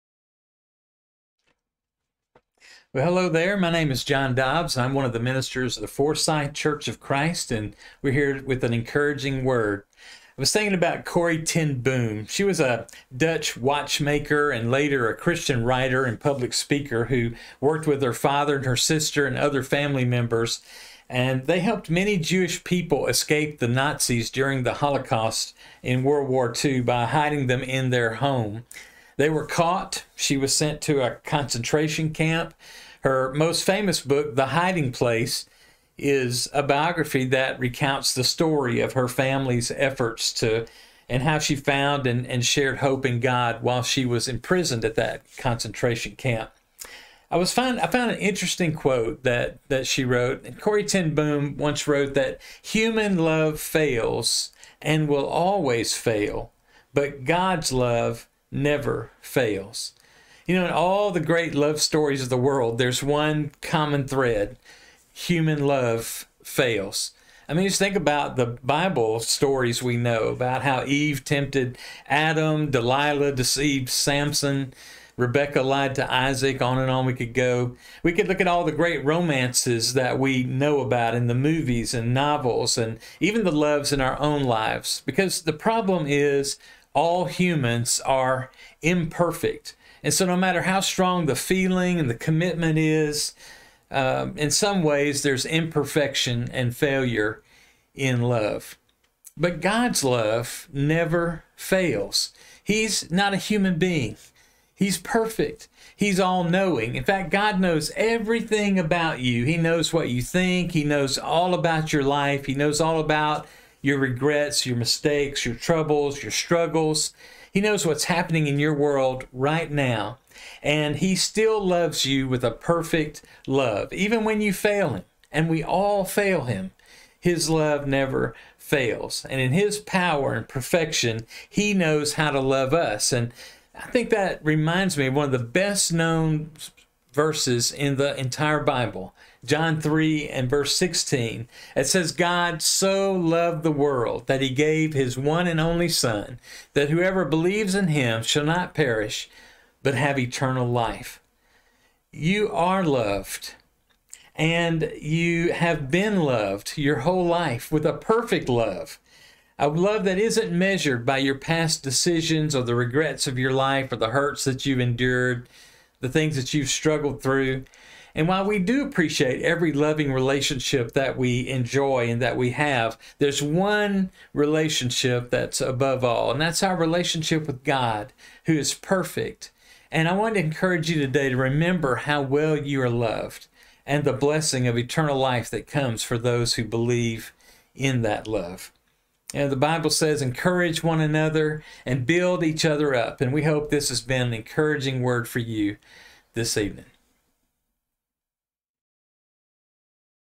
John 3:16 - A Word of Encouragement
"For God so loved the world that..."
John Dobbs has a word of encouragement from this well known scripture.